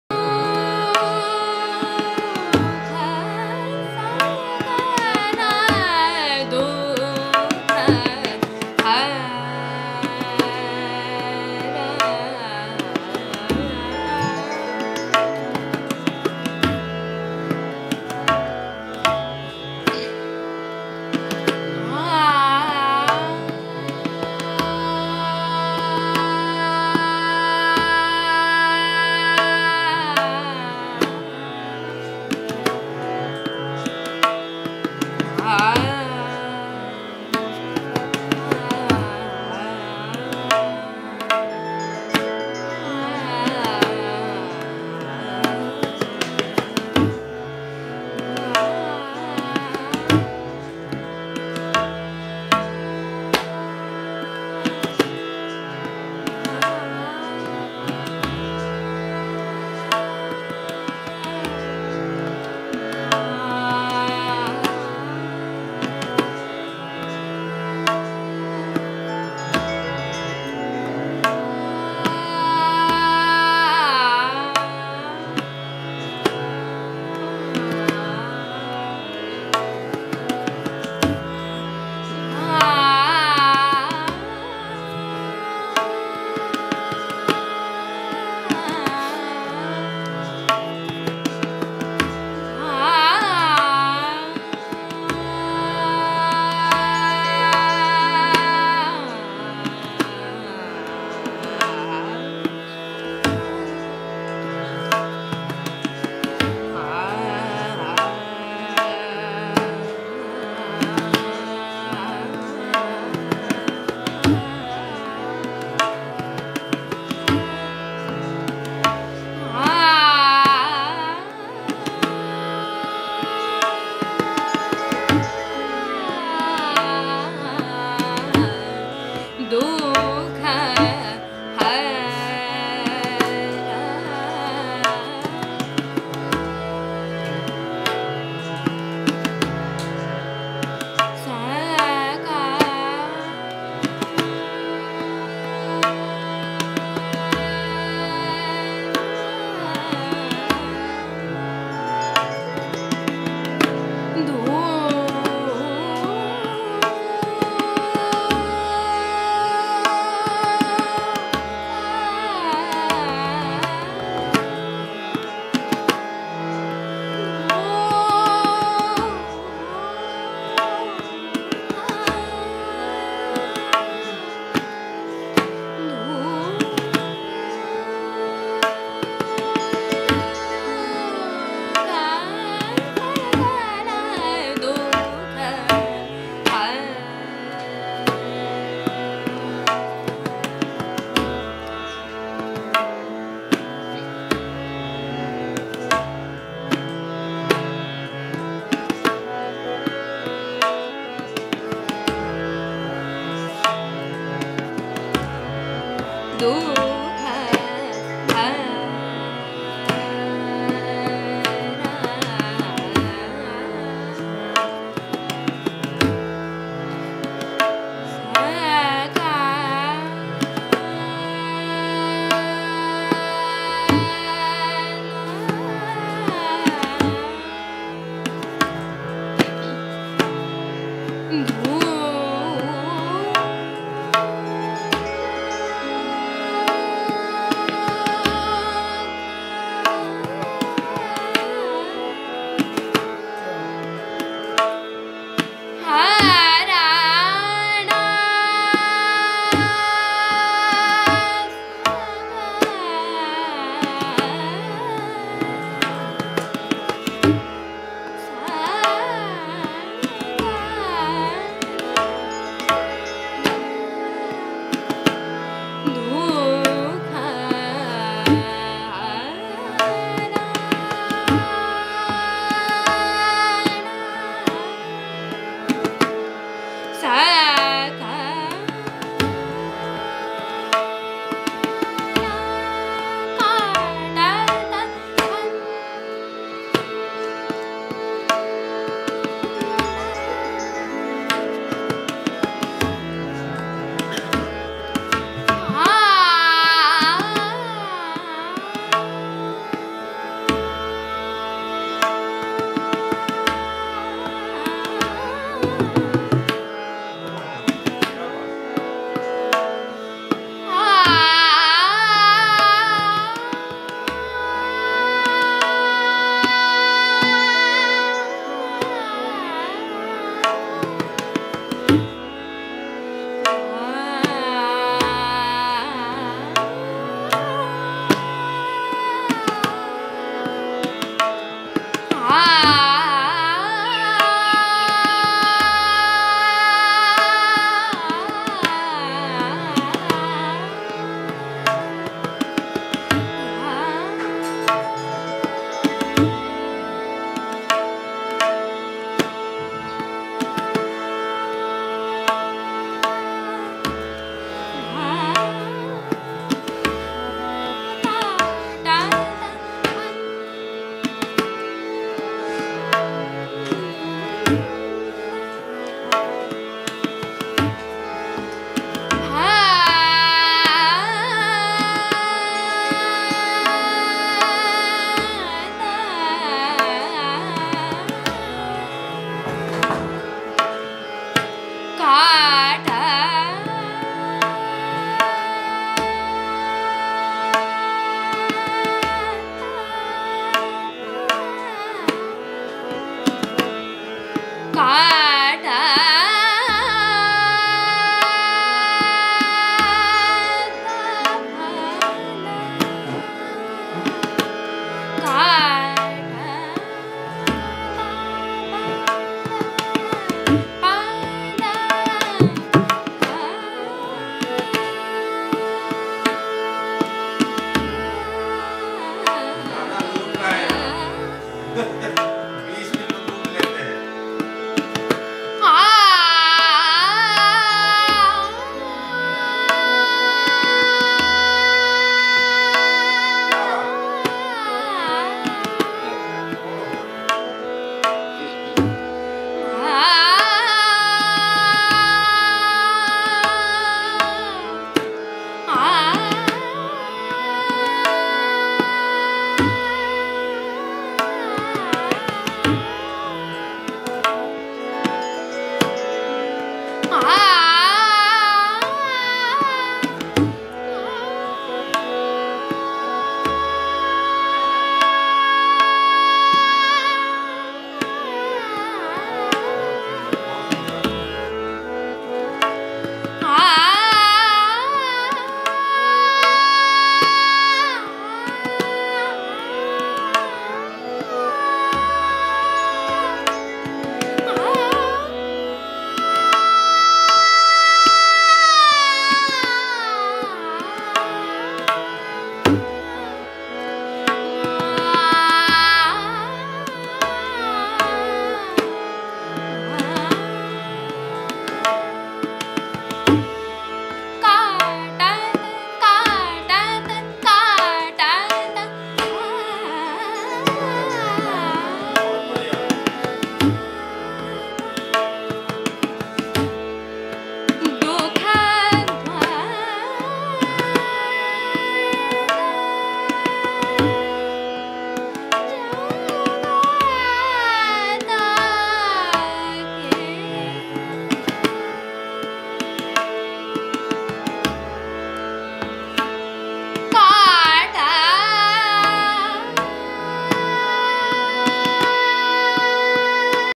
Vilambit khayal || Daliya Mukherjee || Harmonium || Julia Mukherjee |Tabla || Ranjib Biswas
vilambit khayal | taal | Rupak |7 beats | raaga | hanswadhani |
varanasi